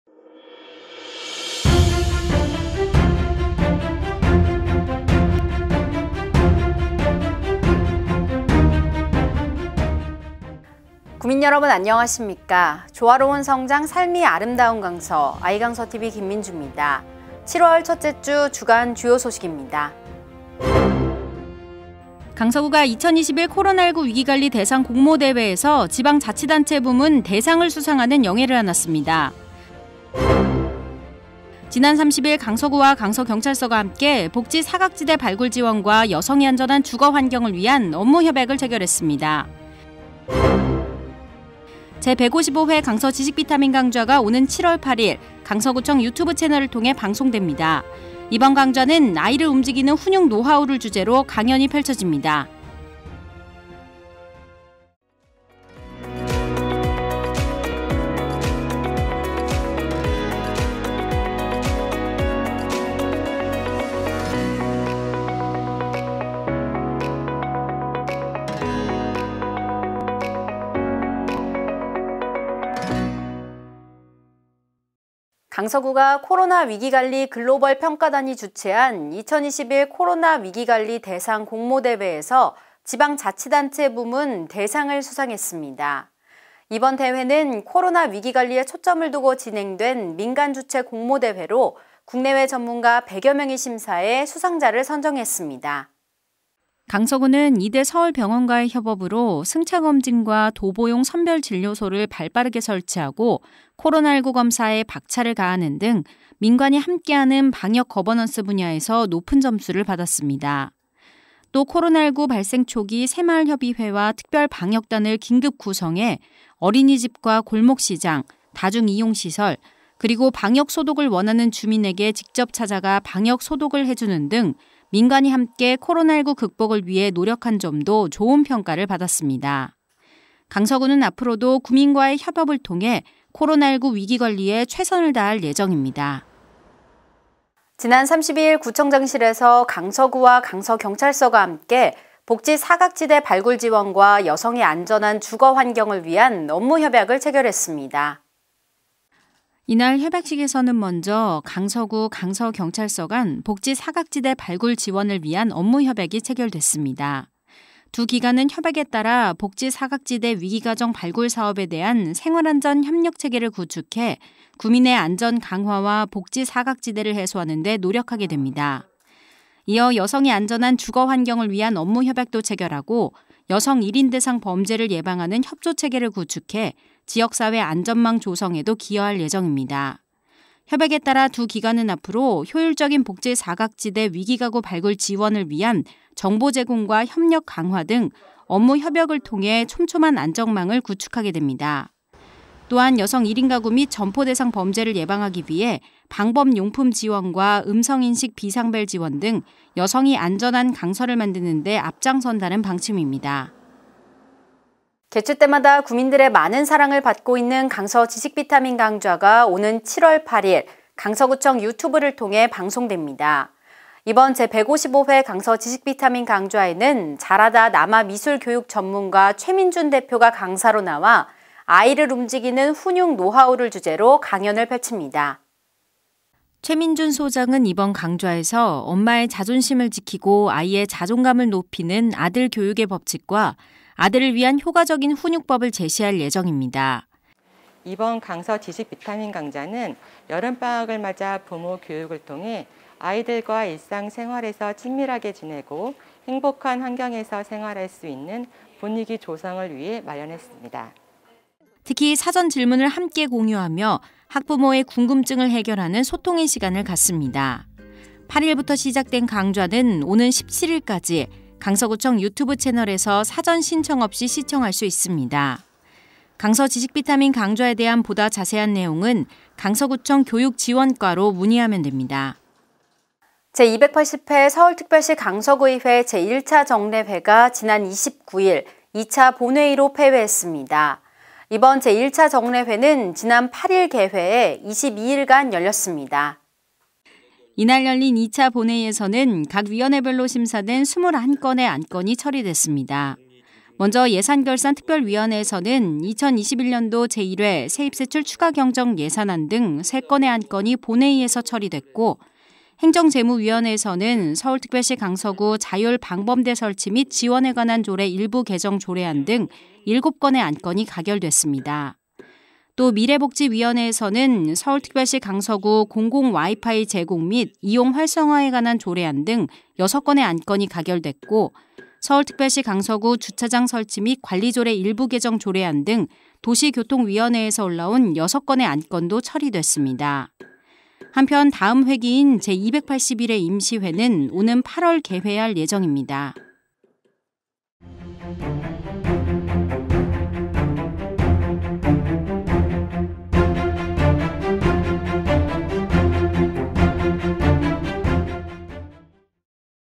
2021년 7월 첫째주 주간뉴스
2021년 7월 첫째주 주간뉴스

1. 강서구, 코로나19 위기관리 대상 공모대회 대상 수상
2. 복지 사각지대 발굴·지원 및 여성 안전 주거환경 구축 업무협약
3. 제155회 강서지식비타민강좌 개최
4. 제280회 서울특별시 강서구의회 제1차 정례회 폐회